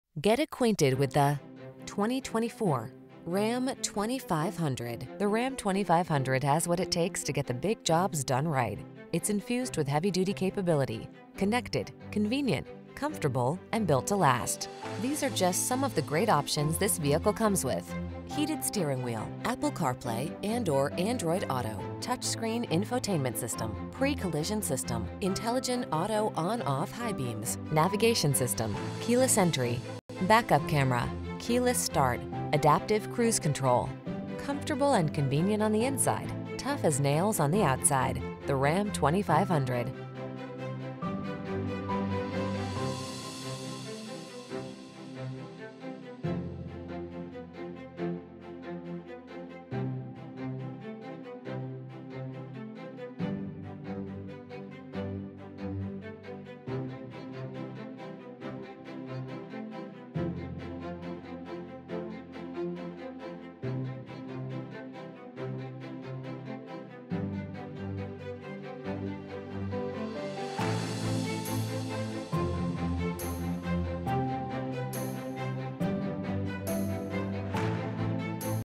2024 Ram 2500 Big Horn Jacksonville, Orange Park, Gainesville, Ocala, Lake City FL
New 2024 Ram 2500 Big Horn available at Murray CDJR located in 15160 US Highway 301 South, Starke, FL, 32091.
Stock#: 330703 - VIN#: 3C6UR5DL4RG330703

Get acquainted with the  2024  Ram 2500. The Ram 2500 has what it takes to get the big jobs done right. It's infused with heavy-duty capability, connected, convenient, comfortable, and built to last. These are just some of the great options this vehicle comes with: Heated Steering Wheel
, Apple Carplay®/Android Auto®, Touchscreen Infotainment System, Pre-Collision System, Intelligent Auto on/off High Beams, Navigation System, Keyless Entry, Back-Up Camera, Keyless Start, Adaptive Cruise Control. Comfortable and convenient on the inside. Tough as nails on the outside. The Ram 2500.